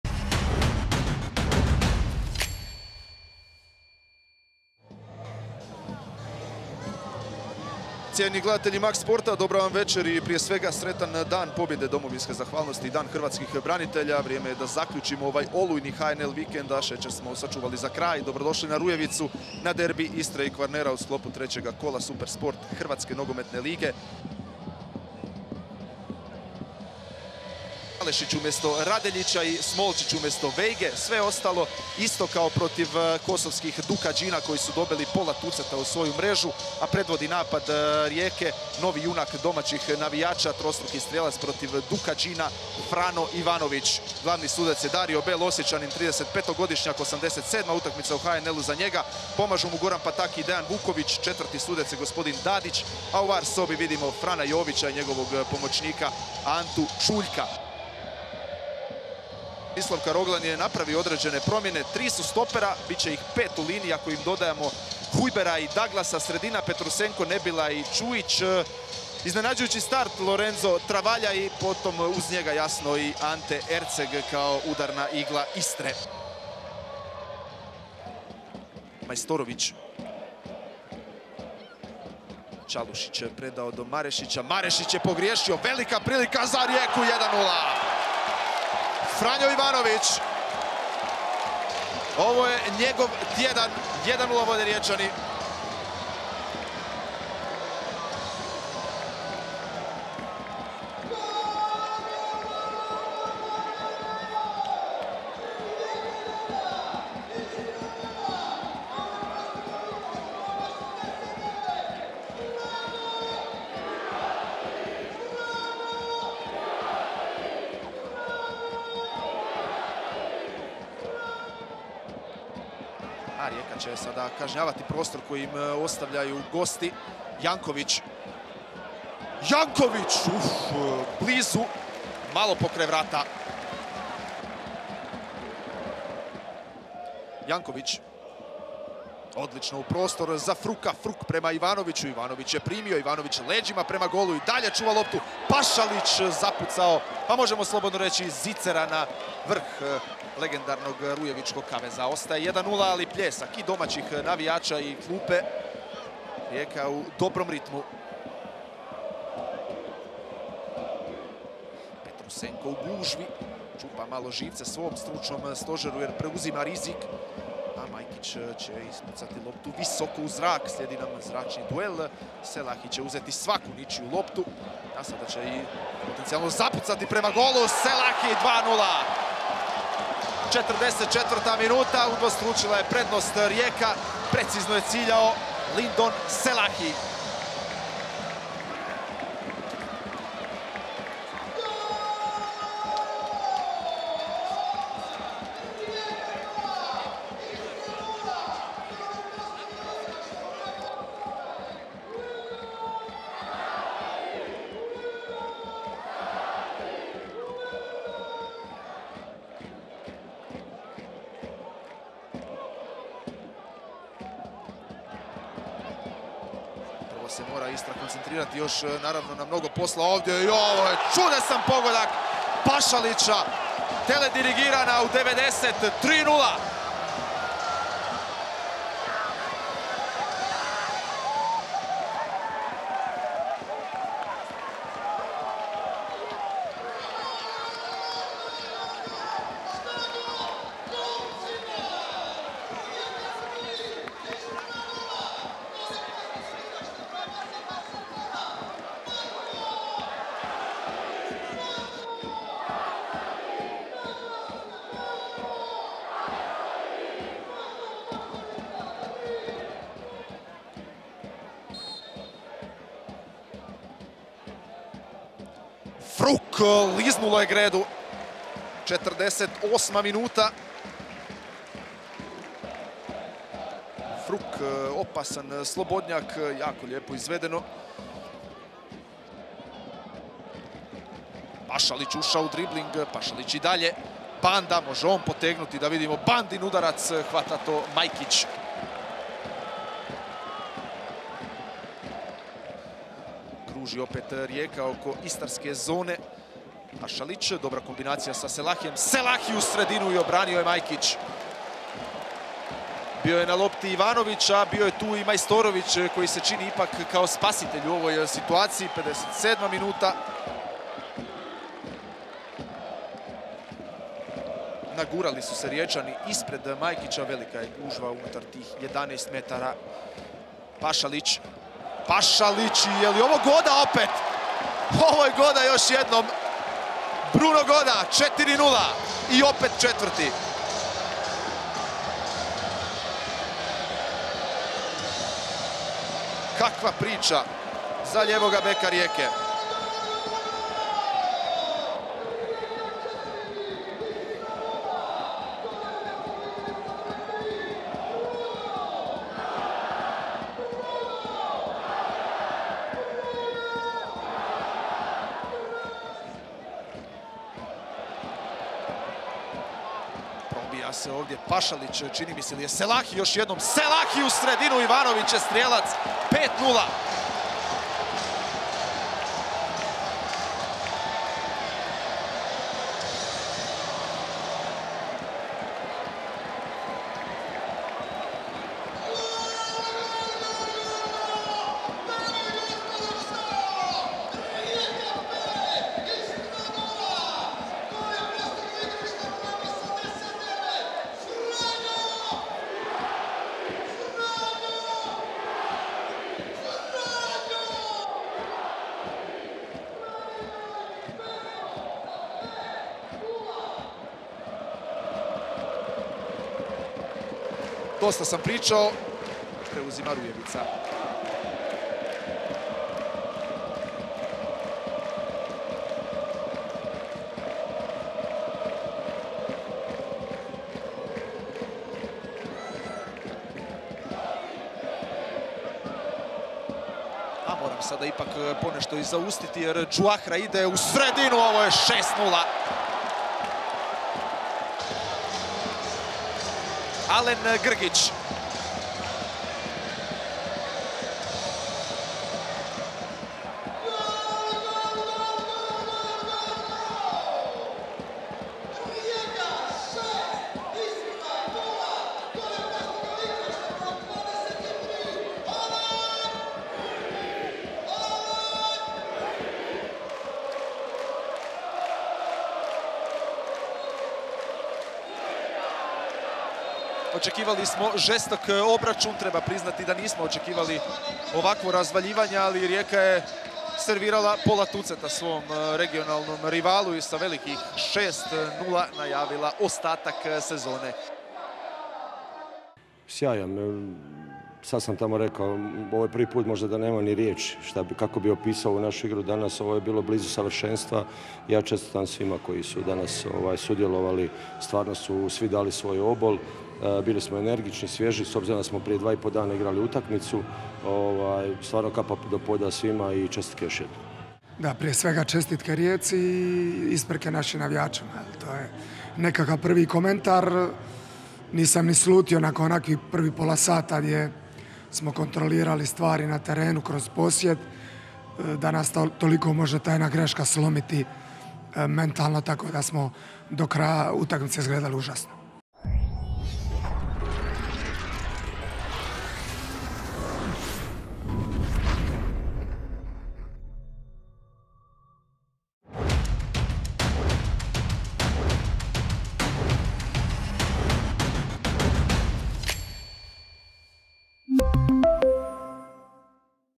RIJEKA vs ISTRA 1961 6:0 (3. kolo, SuperSport HNL 23/24)
Sažetak utakmice trećeg kola SuperSport Hrvatske nogometne lige između Rijeke i Istre 1961 (6:0).